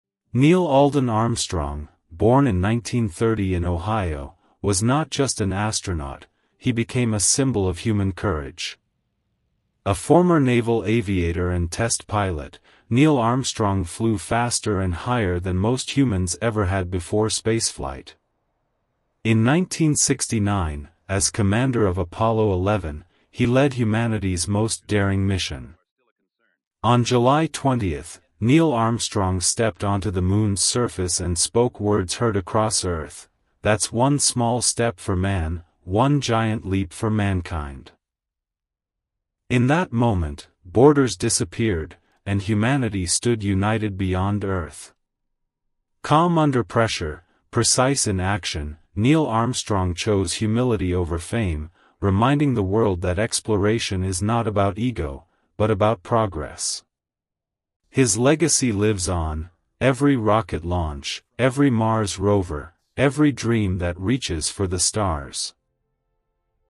Neil Armstrong: The First Human on the Moon.
Step back to 1969 and witness the legendary journey of Neil Alden Armstrong, the first human to walk on the Moon. From his early days as a naval aviator to commanding Apollo 11, this 60-second documentary captures the courage, precision, and humility of a true pioneer. Experience the moment that united humanity beyond Earth and inspired generations to reach for the stars.